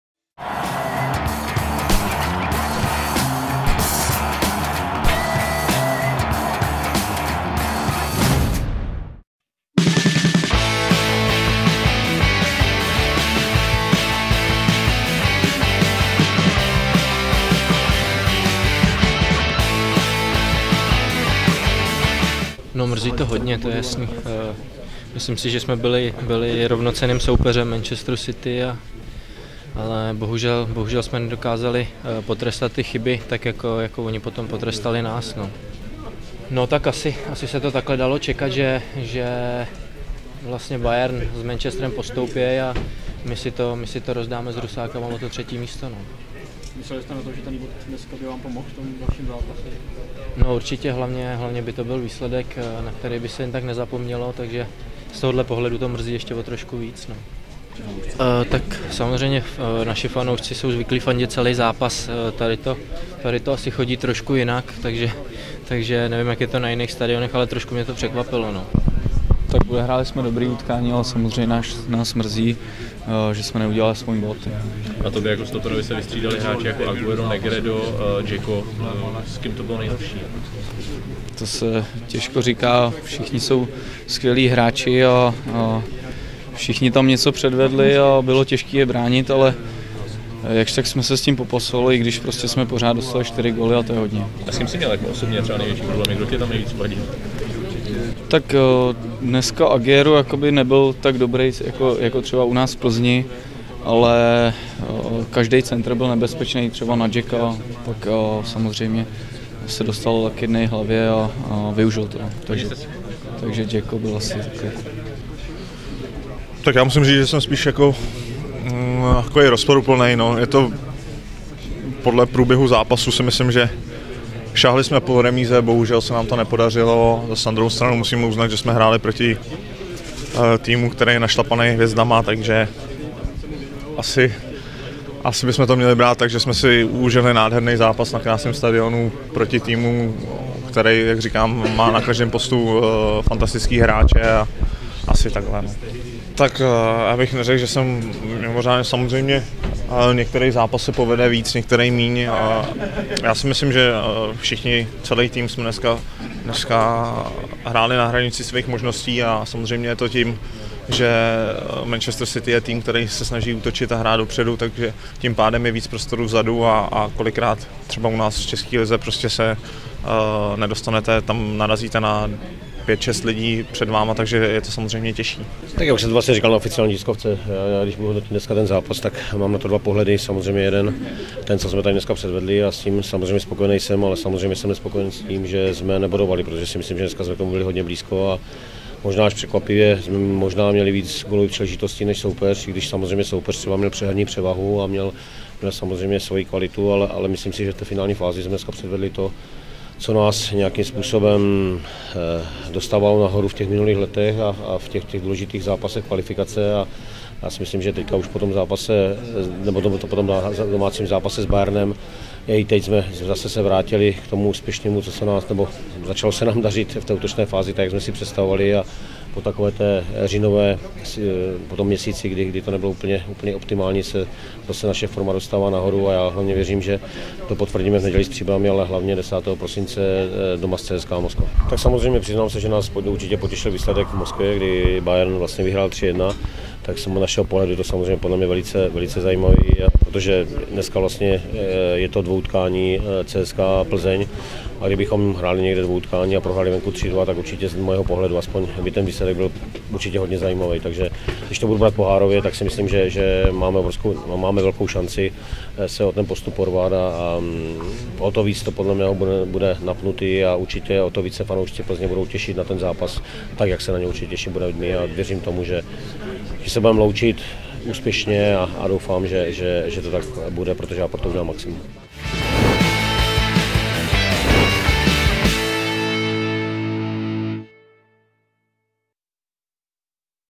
Manchester City FC - FC Viktoria Plzeň 4:2 | 27.11.2013
Poslechněme si ohlasy Viktoriánů po pátém zápase základní skupiny Ligy mistrů, po zápase, v němž byli zdatným konkurentem miliardářského velkoklubu z Manchesteru.